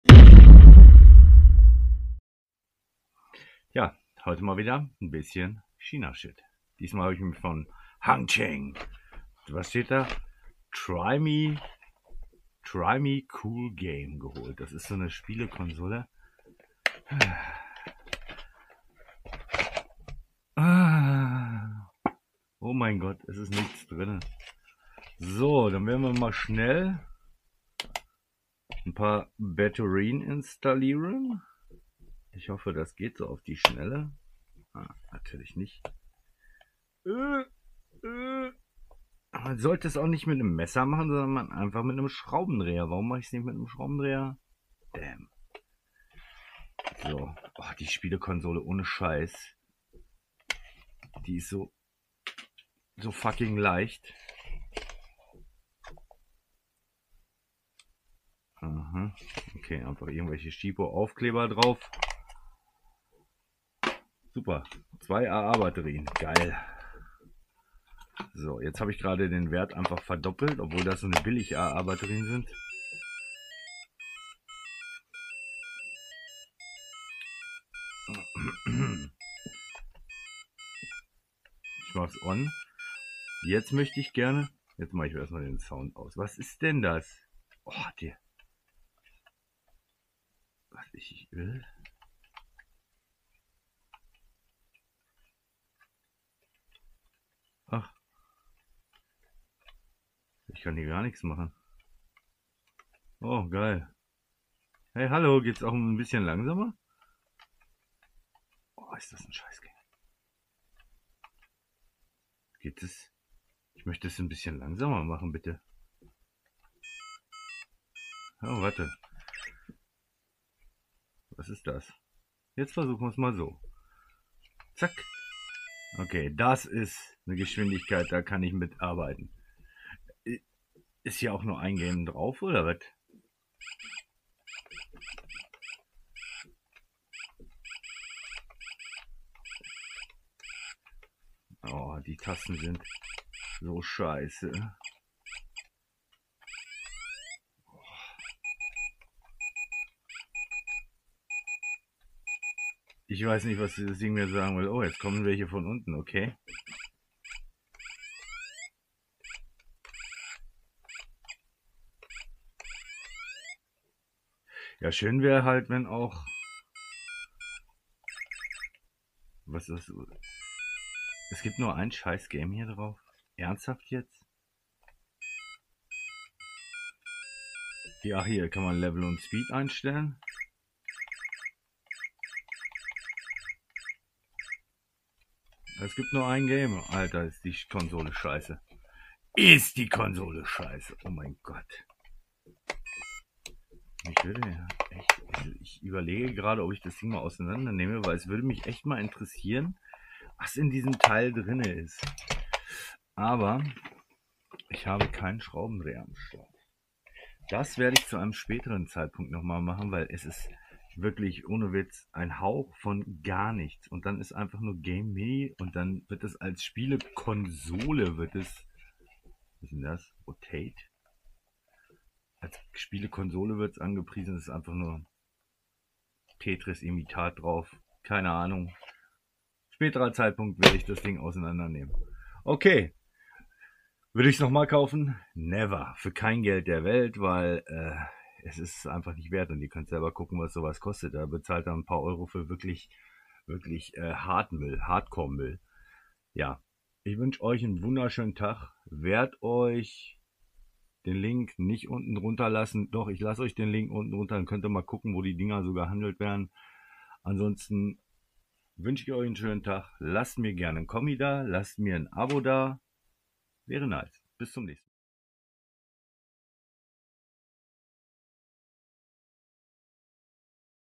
" Cool Game " Spielekonsolen von Han Cheng 👾CHINASHIT Unboxing & Eindruck 👾
Link:
Sorry so ein OberTrash kann und möchte ich nicht verlinken xD

Unboxing Equipment:
Insta360 Actioncam:
Rollei Hear Me Pro Mikrofon:
Gaming Equipment: zu
Maus Roccat Kone: um